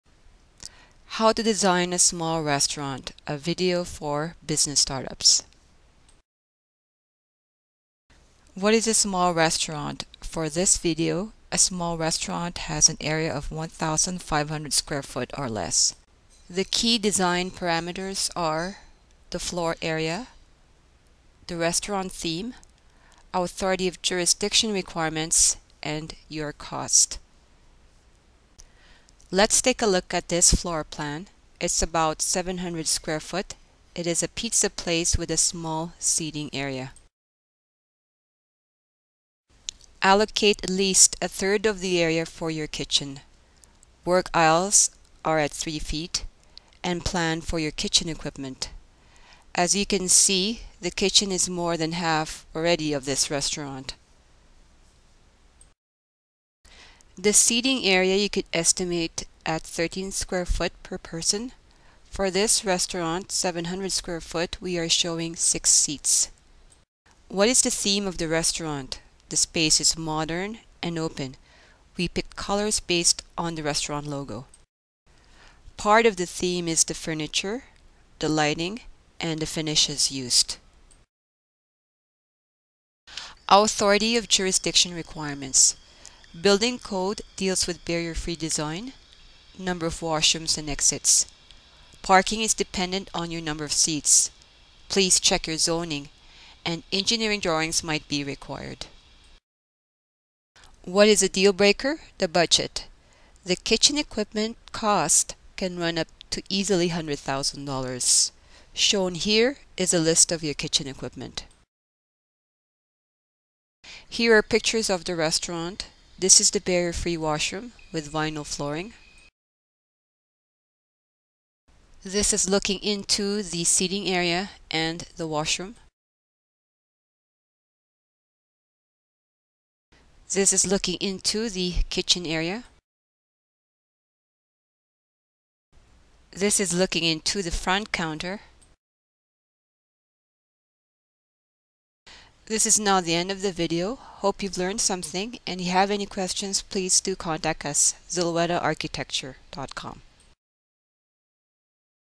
How To Design A Small Restaurant by Zulueta Architecture
Design Tips for Small Restaurants, 1500 SF and Business Start-ups from Calgary Architect, Zulueta Architecture,